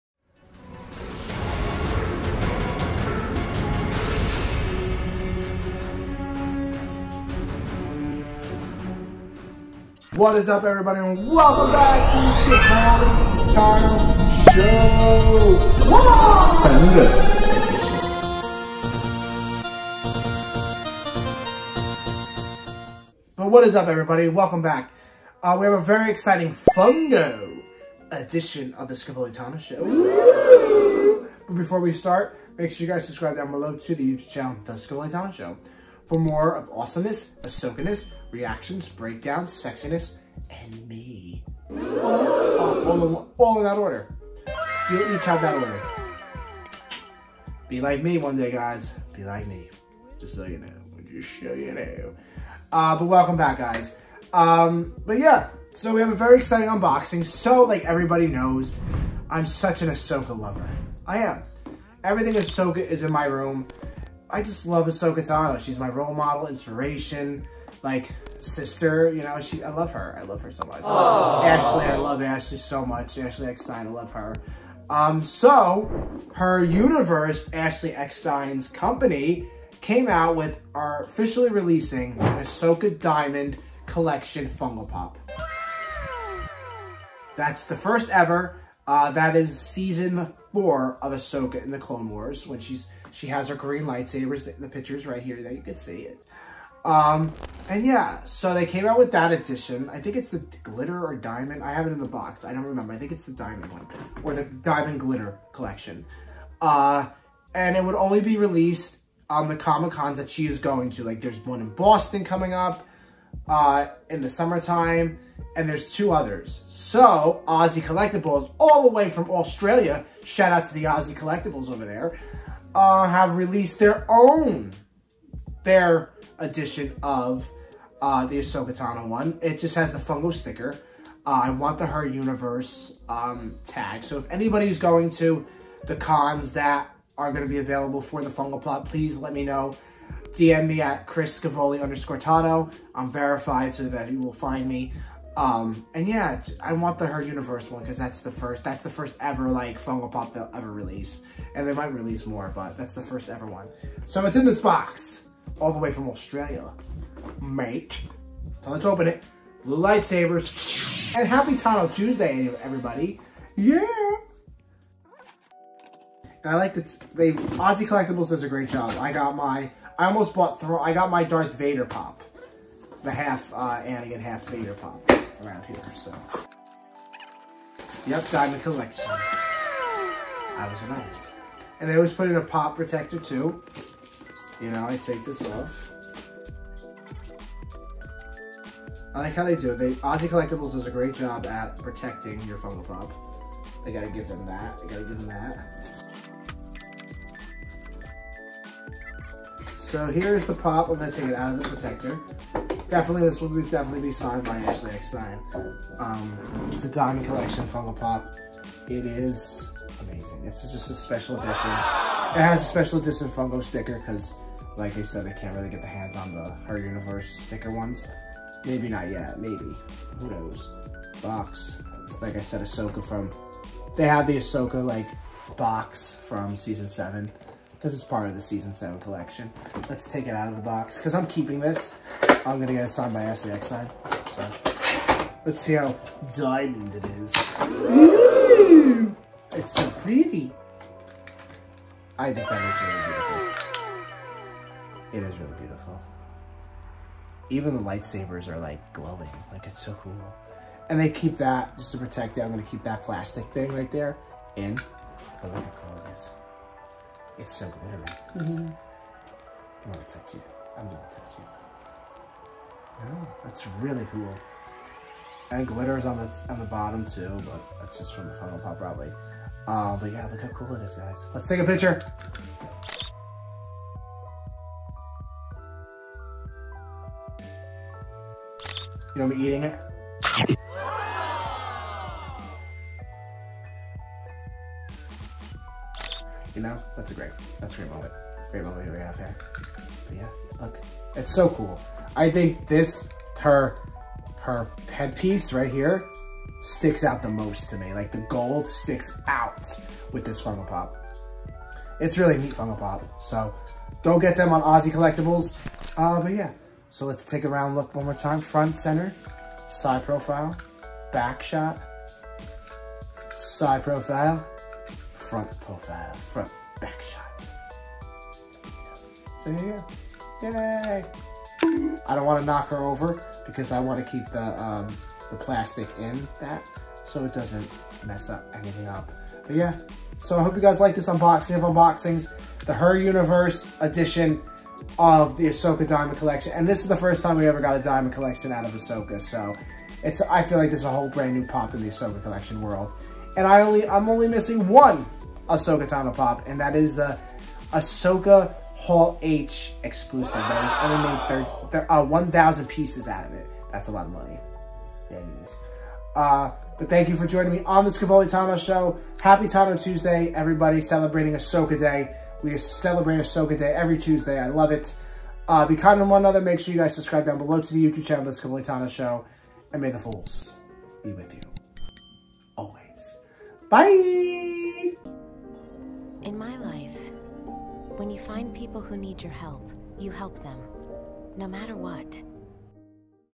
Ahsoka Tano 268 Diamond Collection HER UNIVERSE EXCLUSIVE FUNKO POP UNBOXING!!!
Happy Tano Tuesday everyone!! One of the most important days of the week! I unbox the her universe exclusive ONLY Diamond Ahsoka tano funko pop you can only get this funky at comic cons were Ashley is going and her company is there! I got this funko from an Australia company that sells funkos and it has the funko sticker limited edition on it!!